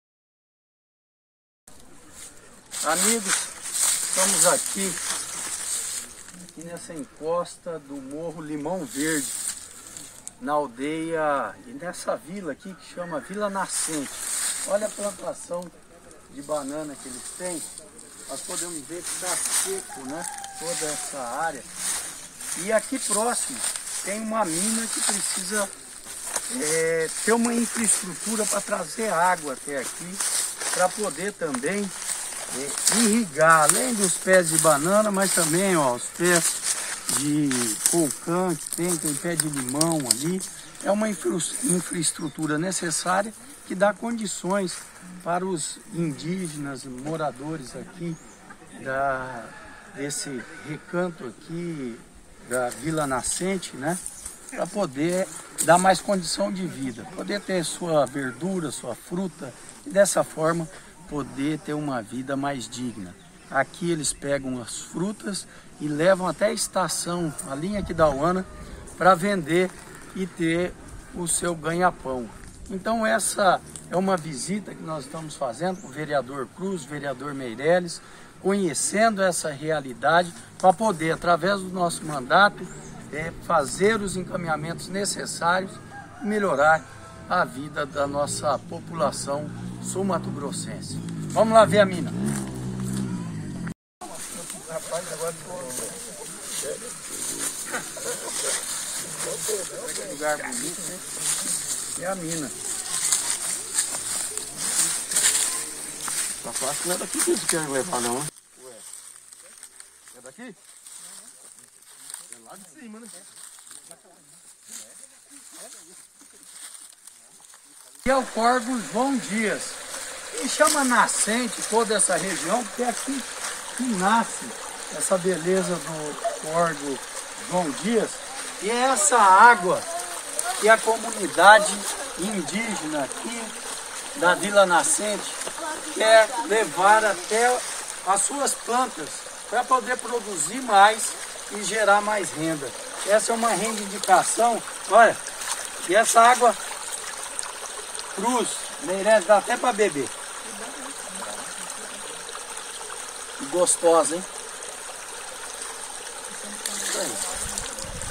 Visita a comunidade Limão Verde, em Aquidauana.
A água é recurso natural indispensável ao desenvolvimento de qualquer produção. Apoiando este Acesso à comunidade Limão Verde, em Aquidauana.